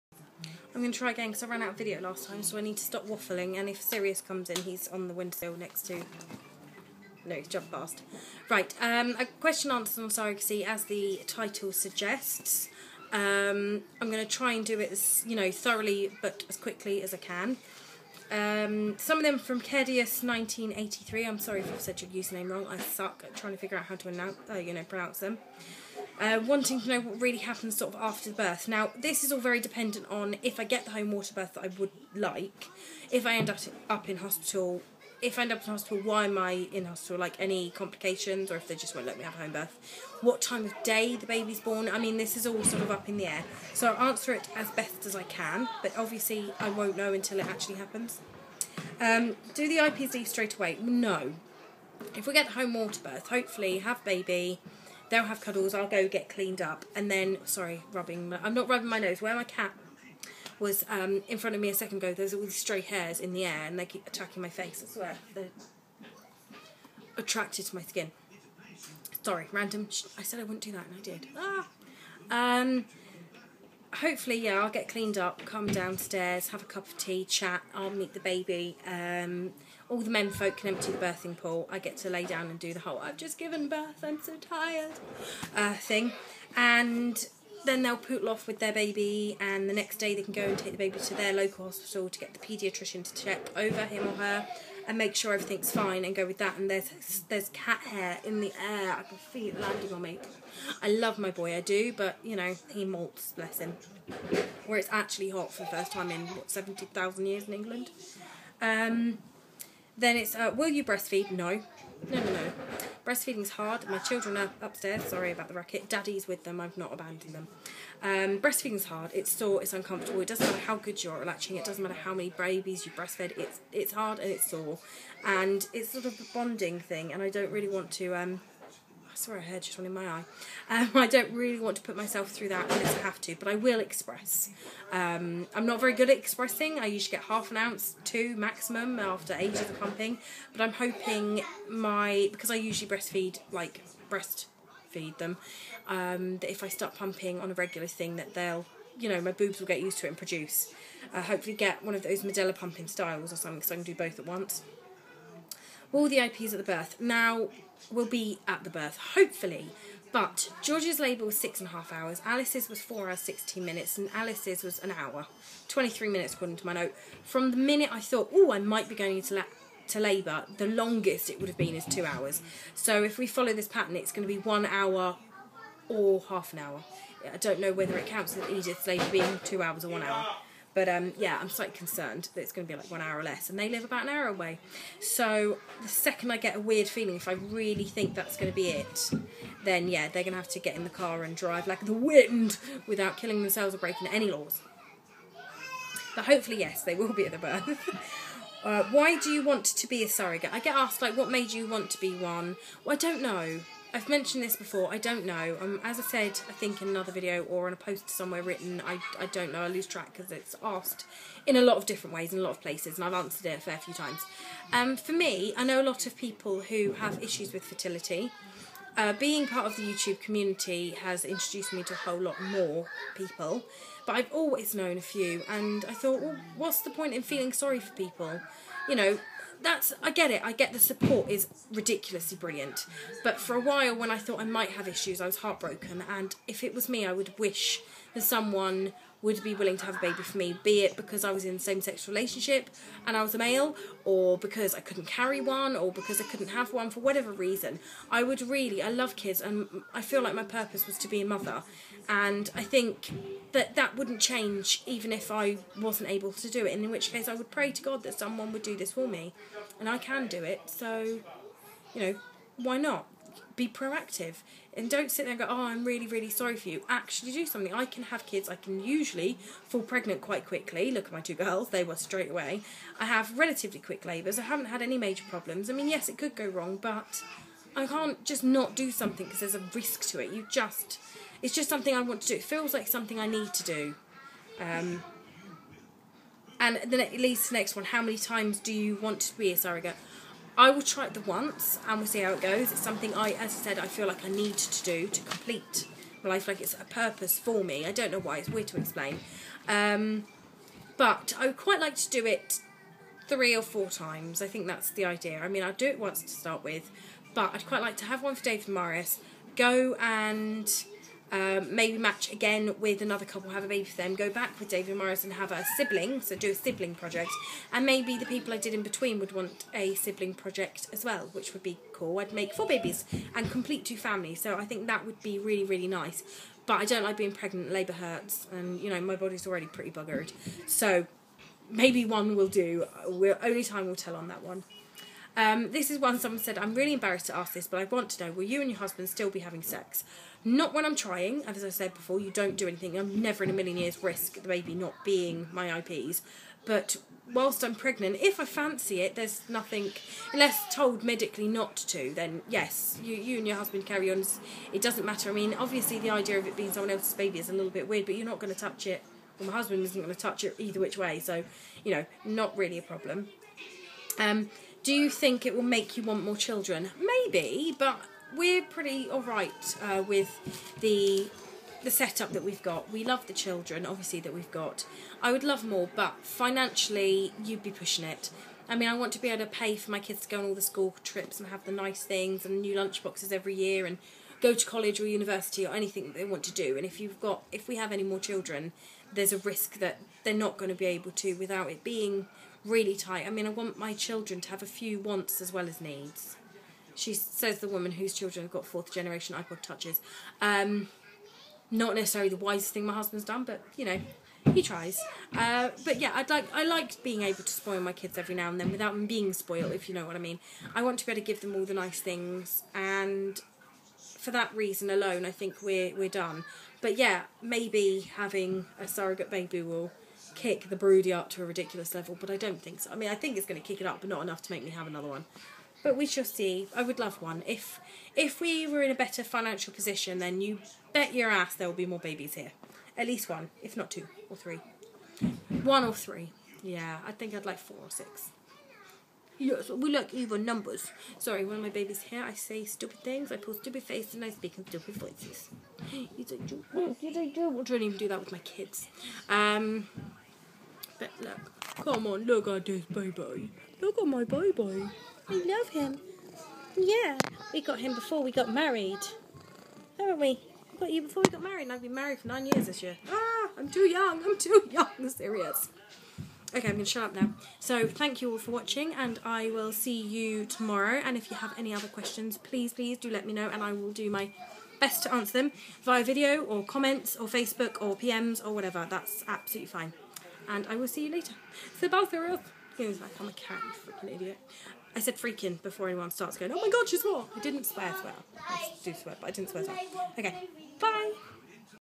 VEDA day 17:Surrogacy; Q&A
Answering a few questions.
If you want to ask anything please comment and I'll happily do a part 2.

The tv is on in the background. I am not advertising in any way nor am I in recipt of any form of payment. No infringement intended.